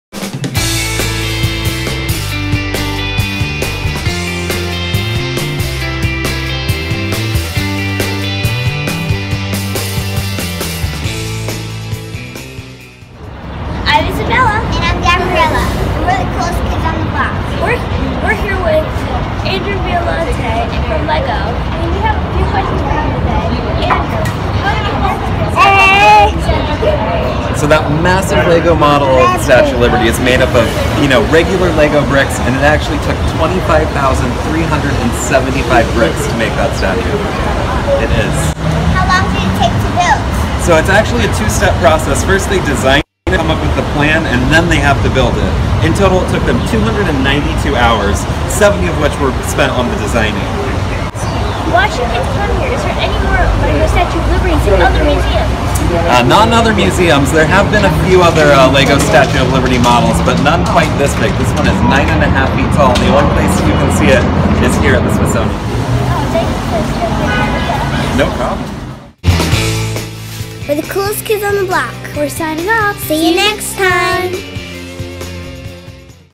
9ft LEGO Statue of Liberty at the Smithsonian
The Coolest Kids on the Block, Isabella and Gabriella, interview Andrew Violante from LEGO Systems, Inc. at the Smithsonian Museum of American History after the unveiling of the 9ft Statue of Liberty and the newest exhibits on display, “American Democracy: A Great Leap of Faith” and “Many Voices, One Nation,” a changing gallery with the inaugural exhibit, “Religion in Early America” and a refreshed presentation of “Within These Walls...,” which showcases 200 years of American history in one house.